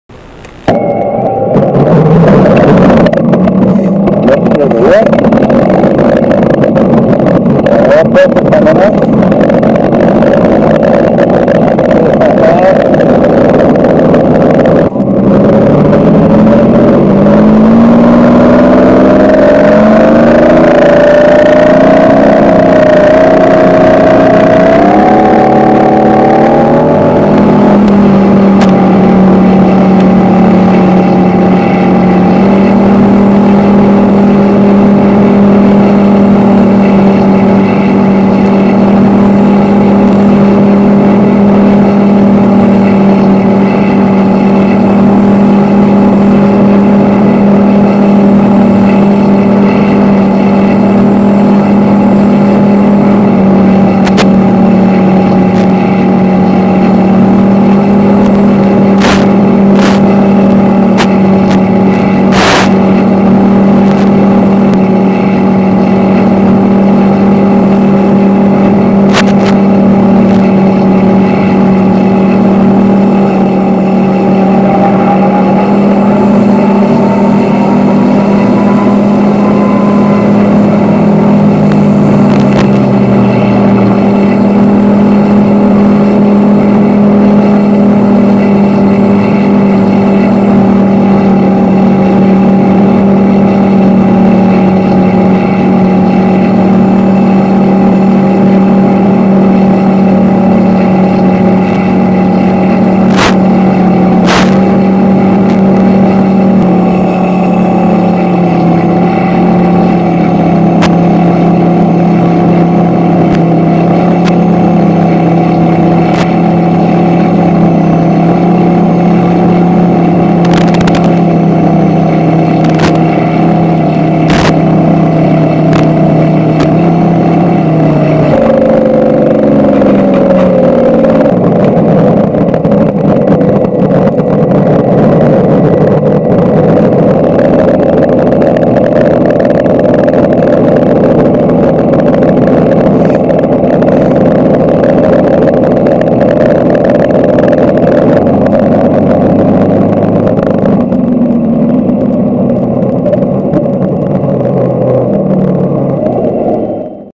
RideSallyRide March 5, 2017
GMARC: March 5th, 2017: Mustang Sally Rides Again! A bit windy.
P-51 RC Warbird, FPV, with my sounds from Macon Belle P-51.  Fatshark Camera, Gimbal, Transmitter, Freewing P51 Mustang, Benedini Sound Board and Amplifier, Tiny Microphone inside the cockpit.